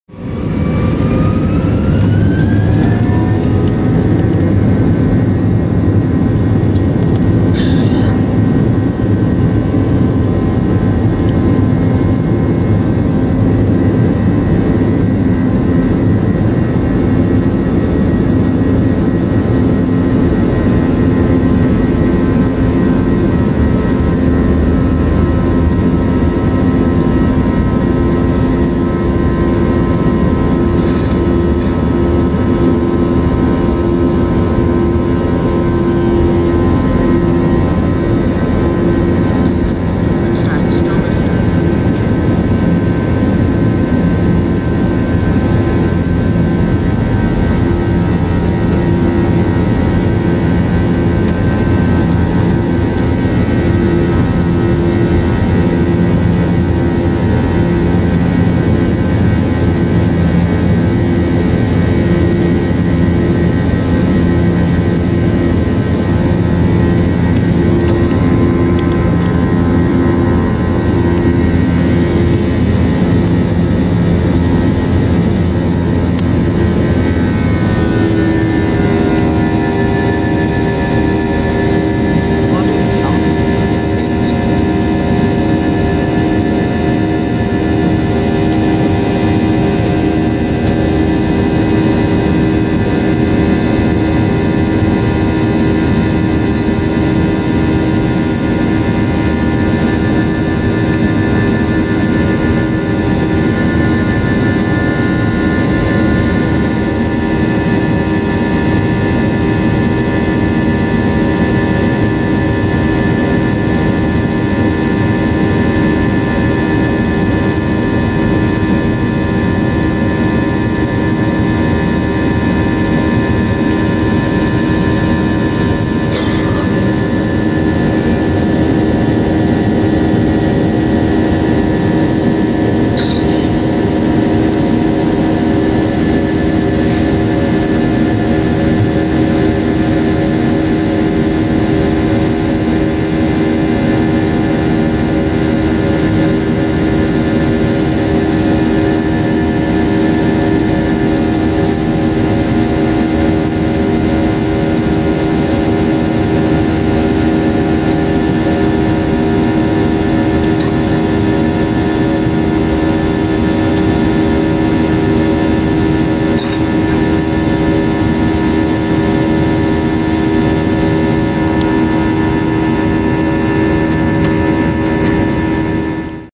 Takeoff from Abu Dhabi! Etihad 283 to Cochin.
Takeoff from (correction 31R not 13R) runway frok Abu Dhabi International Airport on the Etihad A320-200 (Flight 283). Stunning views. Also I do apologise for the slanted camera angle during takoff roll as I was holding the camera with one hand :)